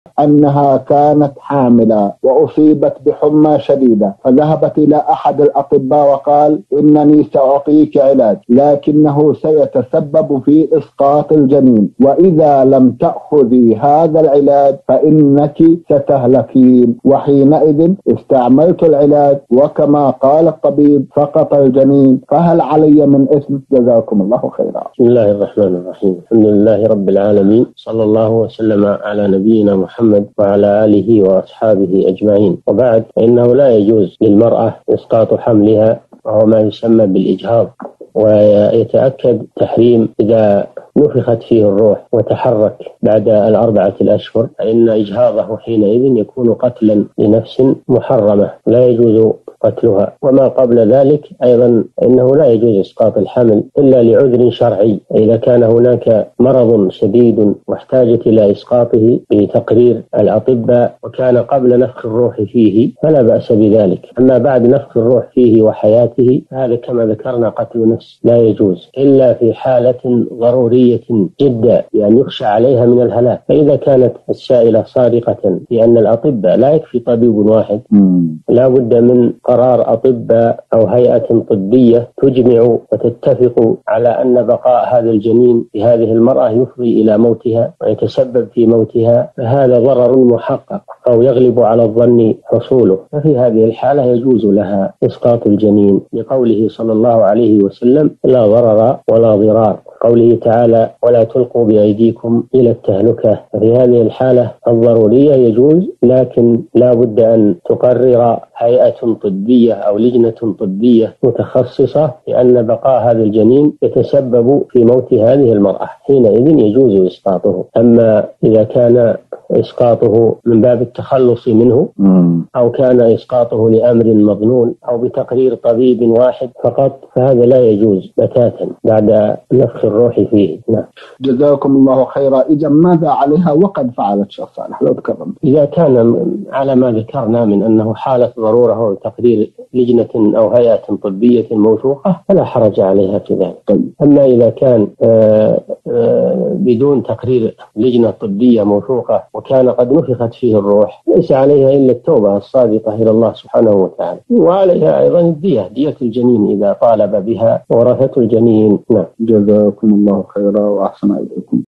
امرأة حامل قال لها الطبيب إن لم تأخذي العلاج فسوف تهلكين علما أن العلاج يسبب إسقاط الجنين فأخذته...؟
نص السؤال:
تقول أنها كانت حاملاً وأصيبت بحمى شديدة وذهبت إلى أحد الأطباء وقال إنني سأعطيك علاج ولكنه سيتسبب في إسقاط الجنين وإذا لم تأخذي هذا العلاج فإنك ستهلكين وحين إذٍ استعملت العلاج وكما قال الطبيب سقط الجنين فهل علي من إثم جزاكم الله خيراً؟
السيرة الذاتية :
للاشتراك في قناة الشيخ صالح الفوزان

رئيس الفريق
لمشروع كبار العلماء